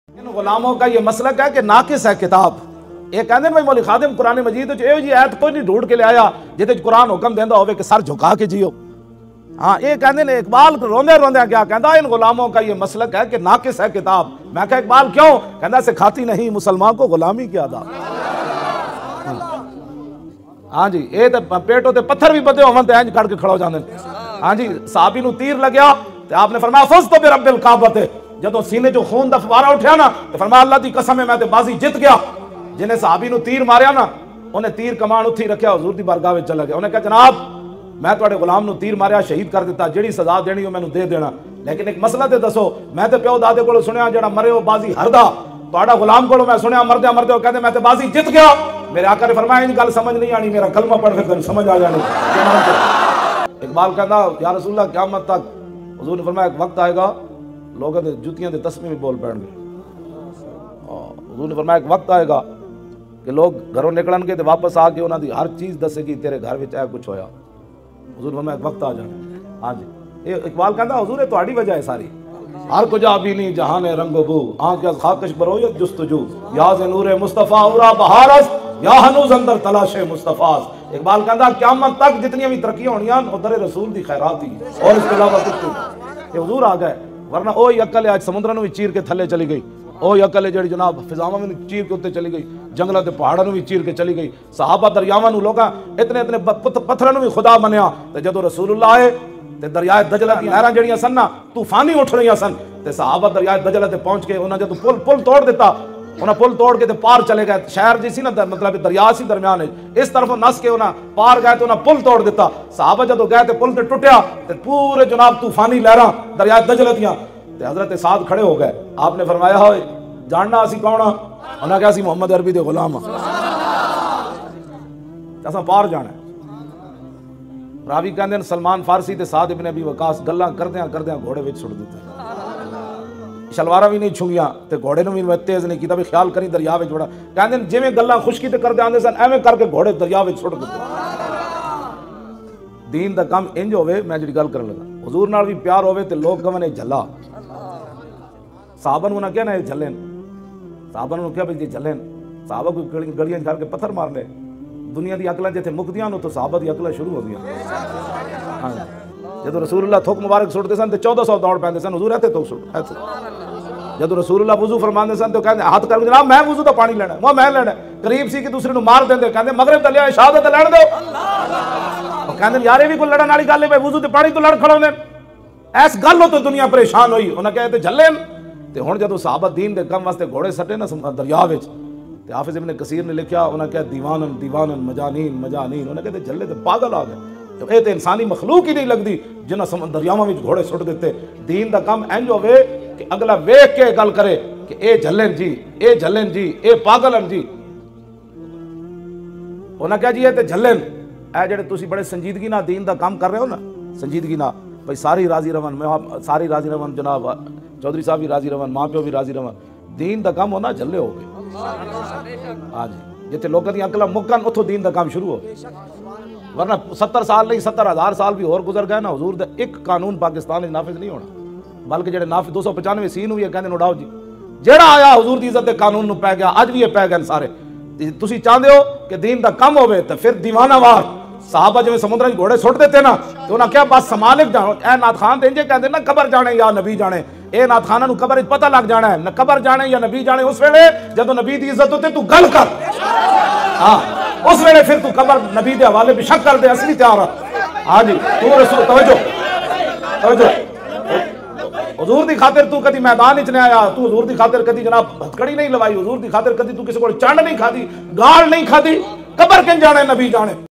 In Ghulamo ka yeh maslaq hai Naqis hai Kitaab - Allama Khadim Hussain Rizvi - Allama Iqbal
In Ghulamo ka yeh Maslaq hai k naqis hai kitab Sikhati nhi Musalman ko ghulami k Adaab